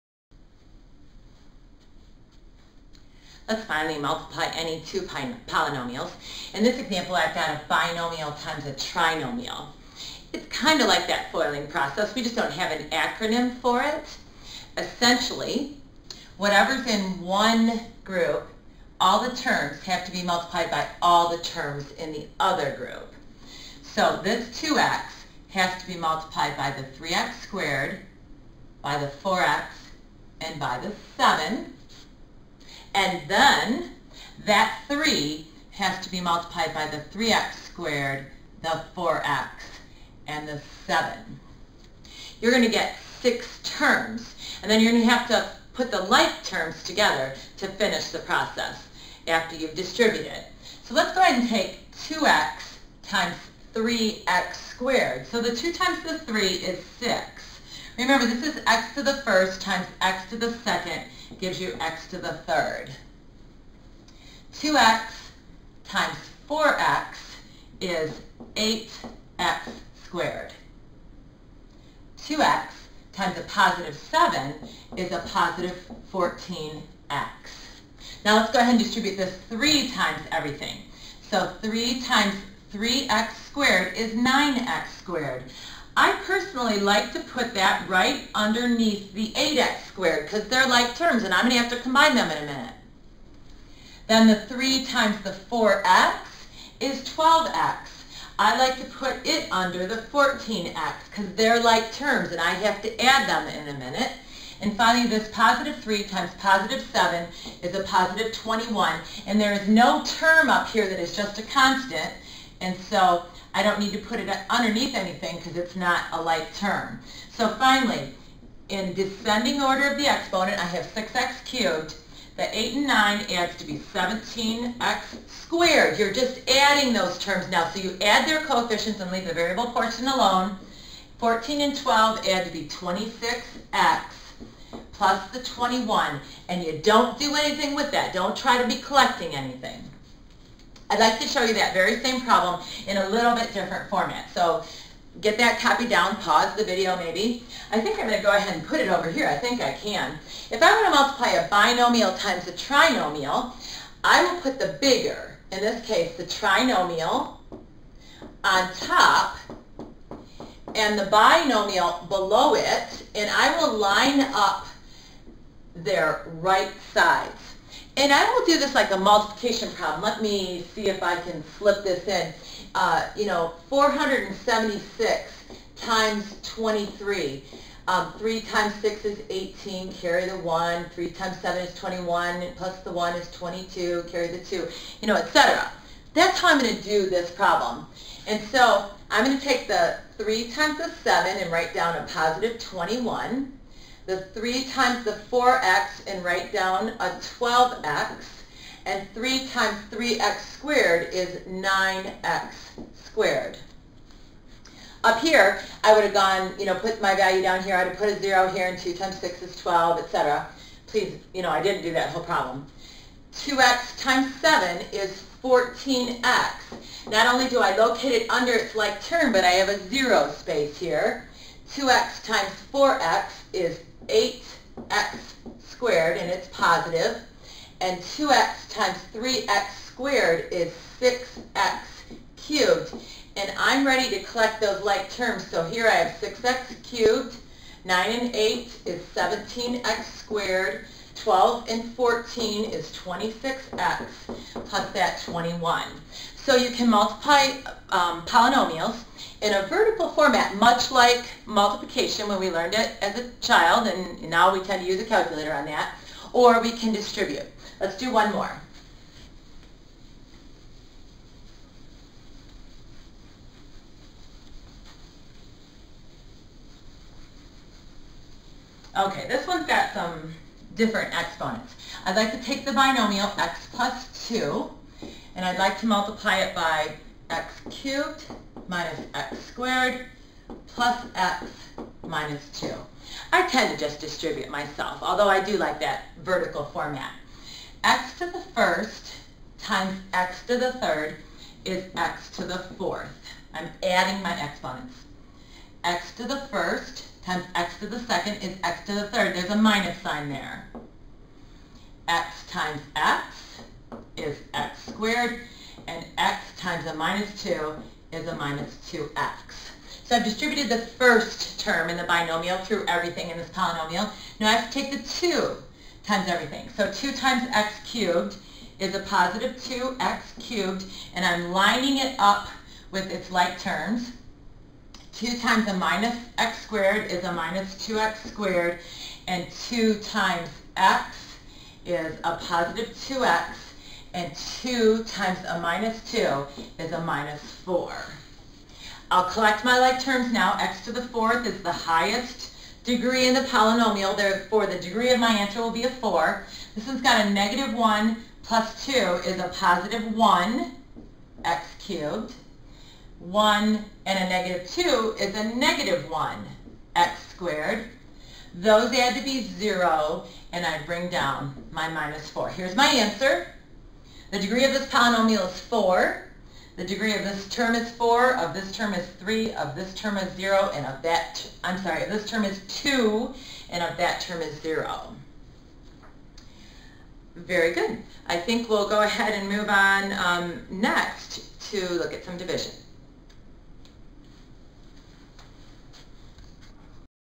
41 - Introductory Algebra - Multiply Polynomials Pt3
Lecture on multiplying polynomials part 3